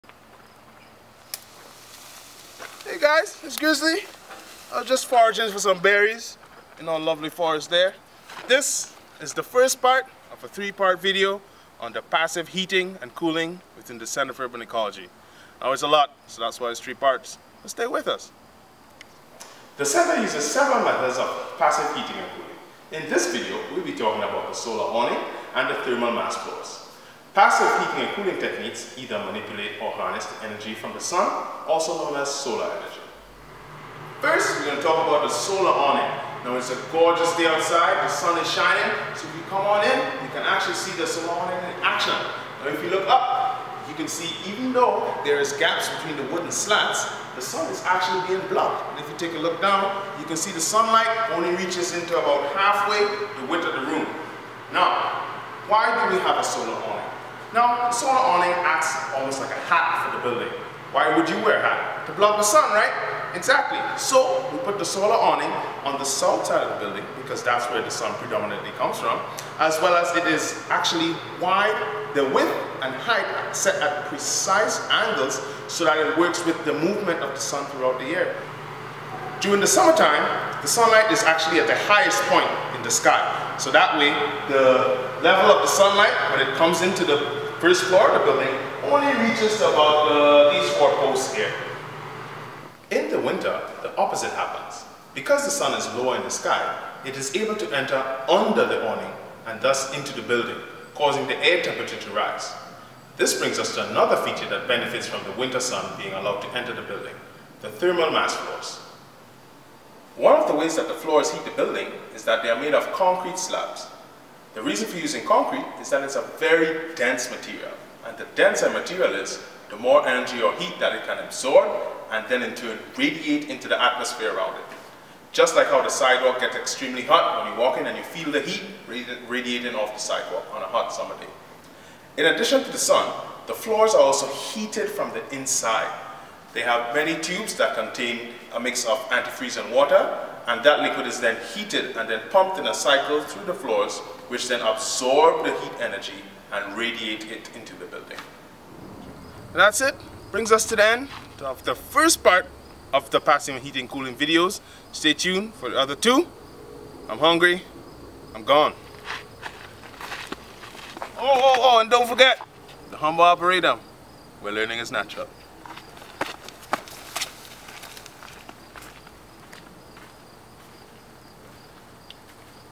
The Centre For Urban Ecology - Building Features - Passive Heating and Cooling - Part 1 of 3
Learn about the passive relationship between the Centre for Urban Ecology and the Sun.